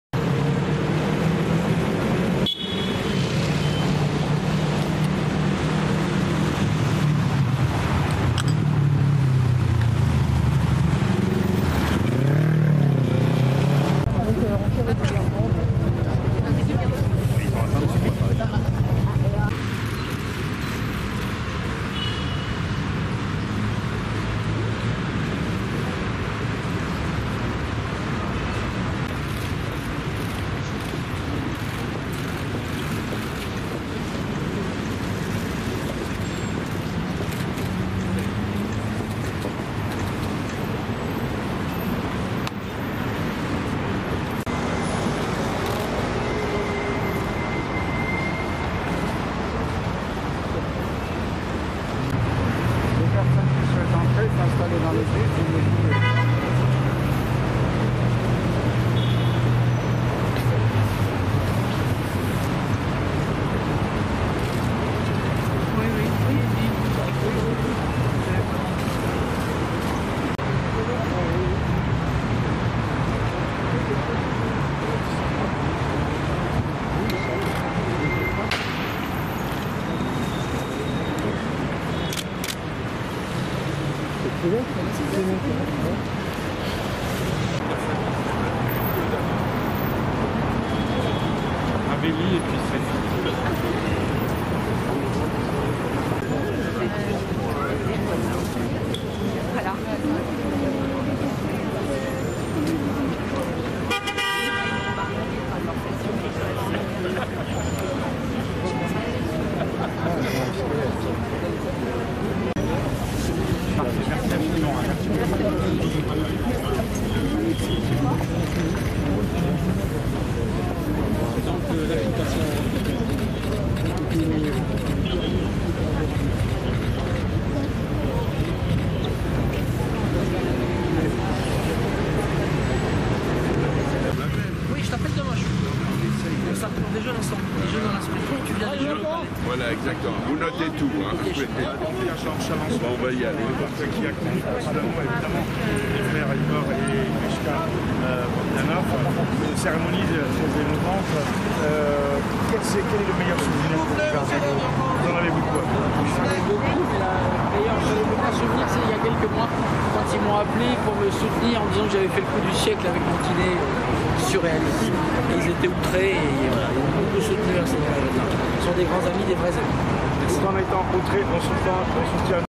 Pierre-Jean Chalençon, Francis Lalanne, Massimo Gargia, etc : obsèques Igor et Grichka Bogdanoff
Pierre-Jean Chalençon, Luc Ferry, Laurence Sailliet, Massimo Gargia, Daniel Lauclair, Georges Fenech et Francis Lalanne étaient présents à l'Eglise de la Madeleine, ce lundi 10 Janvier 2022, pour rendre un dernier hommage aux frères Igor et Grichka Bogdanoff.